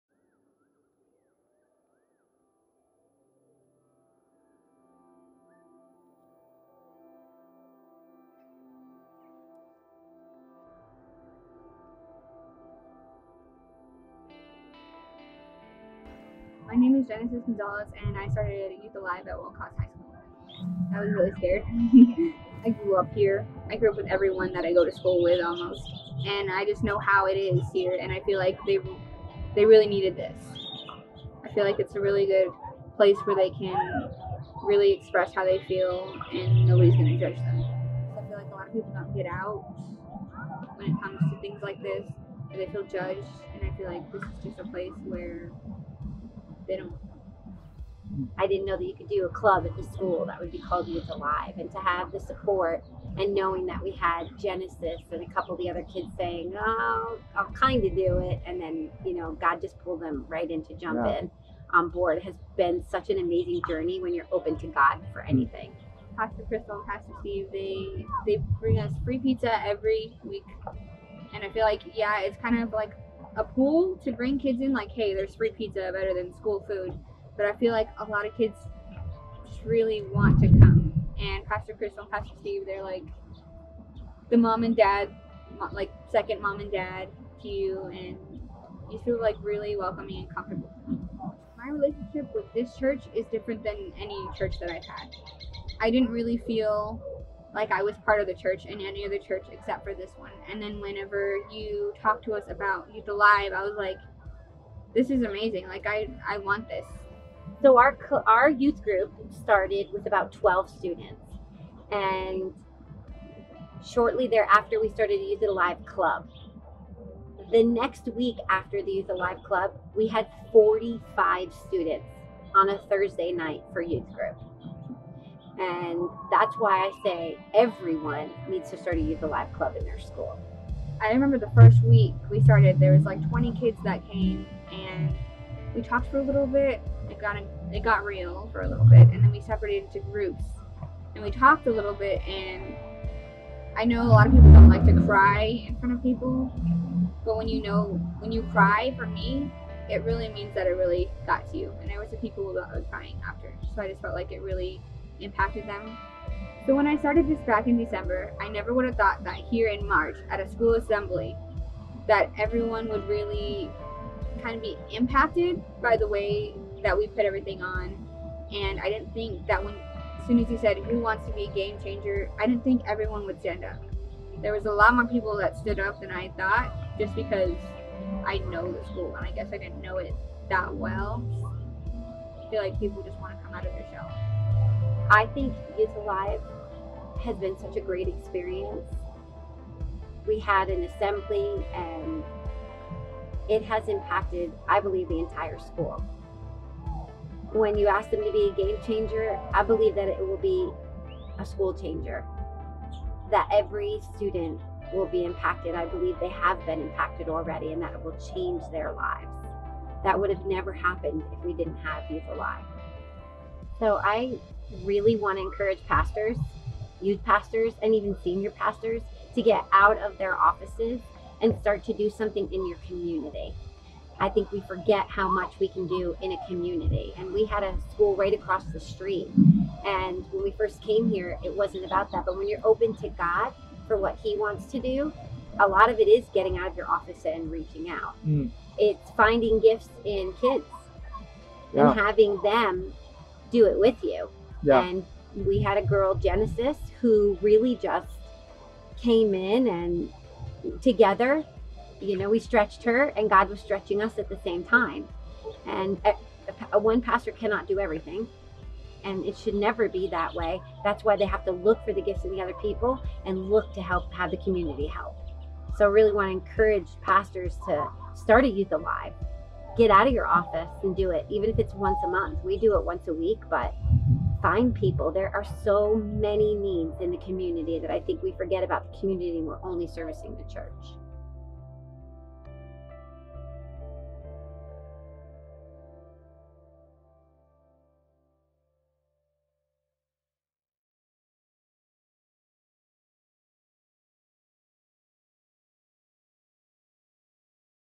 Life-Giving Campus Club Story: Wilcox High School
This is what God is doing in schools all over Arizona!